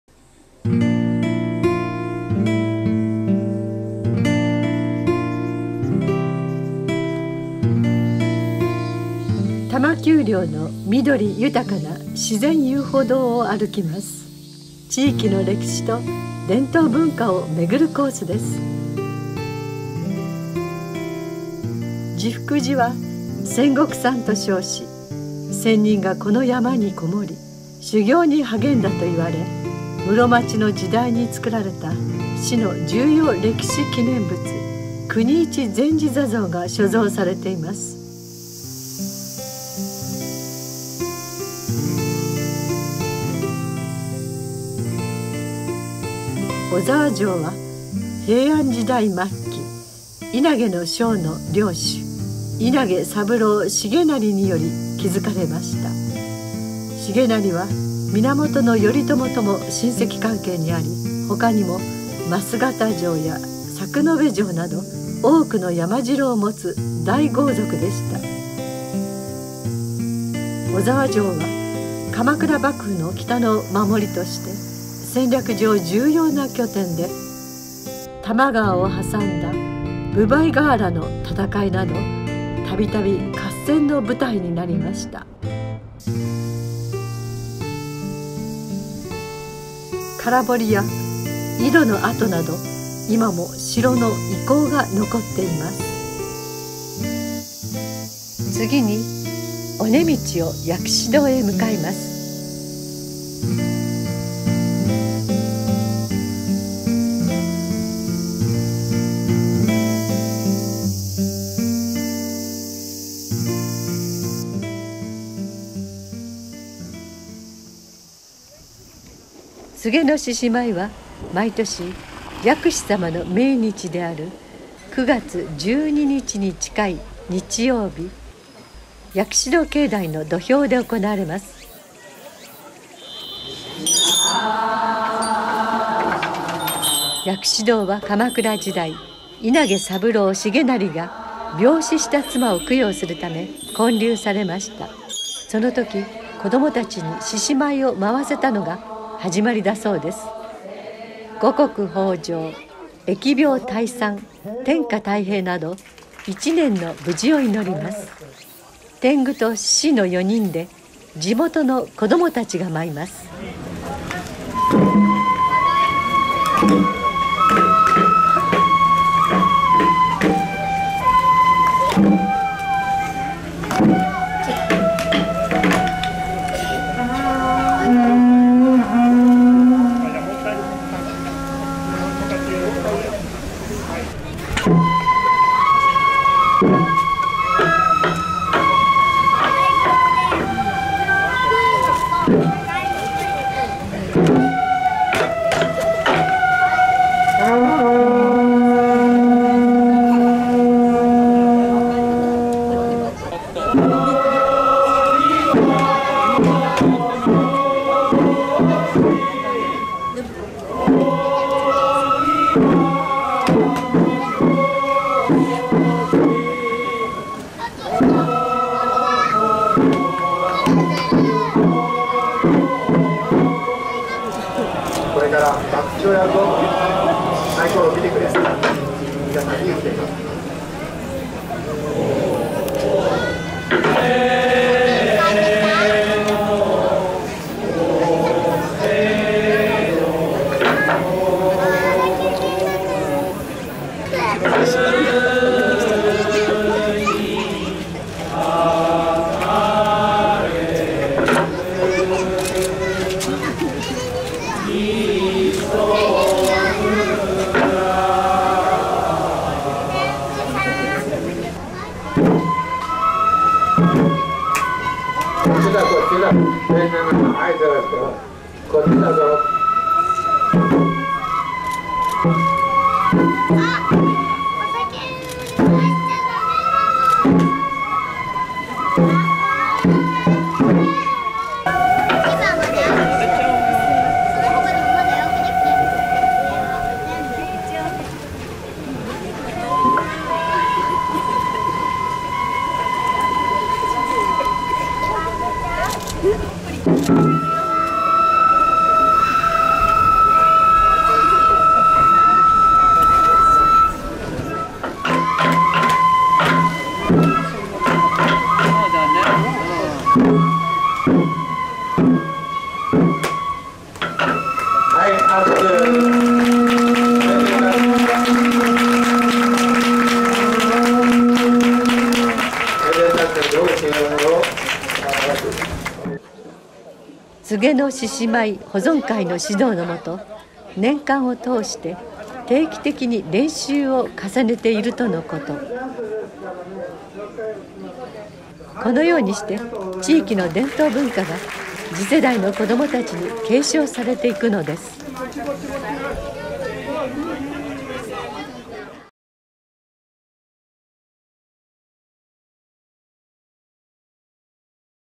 多摩自然遊歩道と菅の獅子舞
多摩丘陵の緑豊かな自然遊歩道（読売ランド前駅→稲田堤駅、5Km）を歩きます。小沢城跡や薬師堂など、地域の歴史と伝統文化を巡ります。

・撮影日：2018年8月10日、9月9日、2019年3月15日
・場所：薬師堂（JR南武線「稲田堤駅」下車 徒歩10分